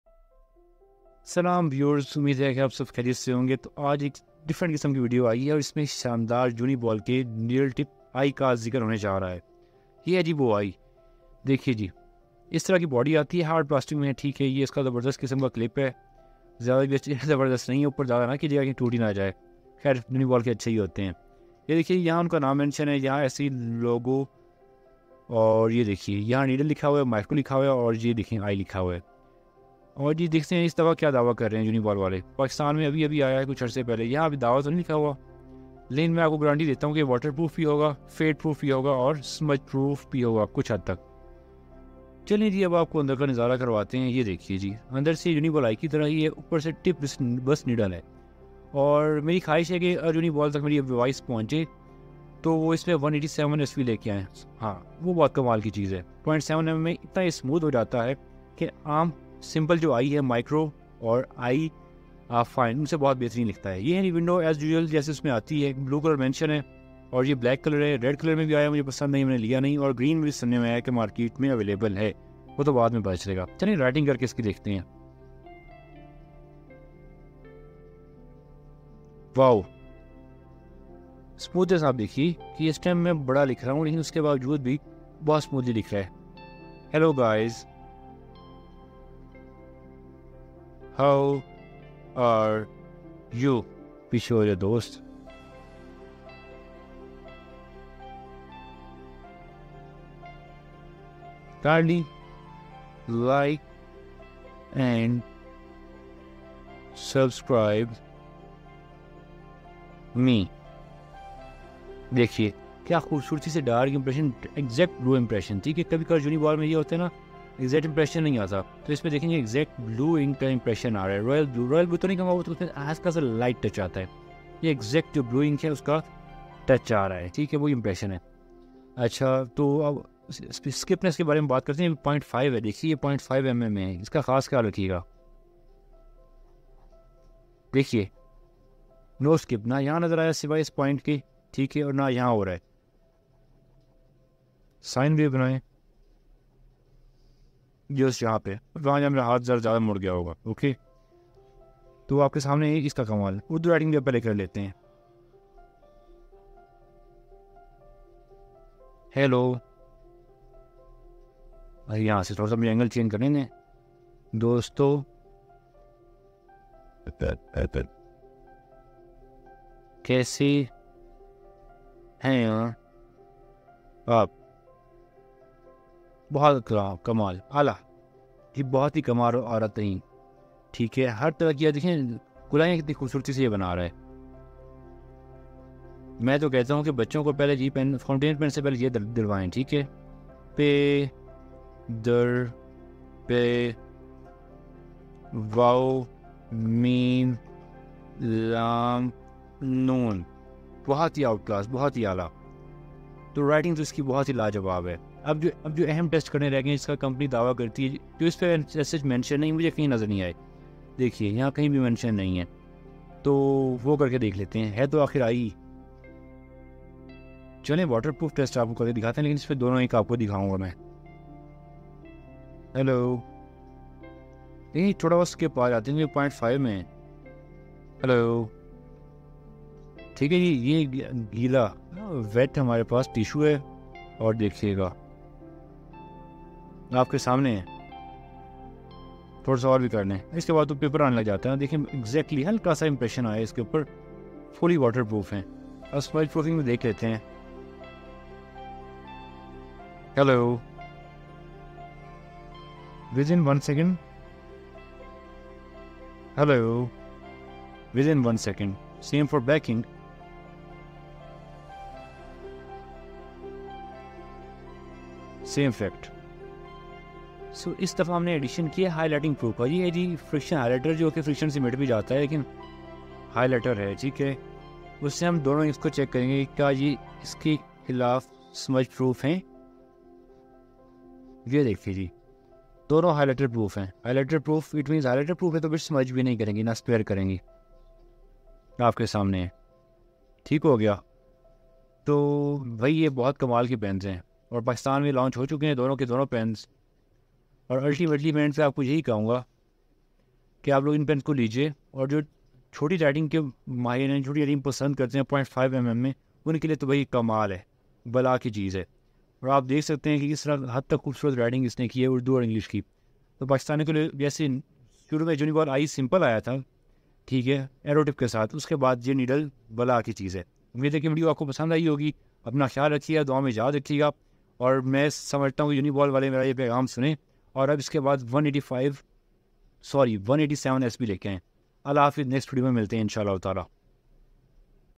Uniball Eye || Needle tip Roller ball pen in Pakistan|| Best roller ballpen with english subtitles.
Uniball Eye || Needle tip Roller ball pen in Pakistan|| Best roller ball pen in this price...with english subtitles..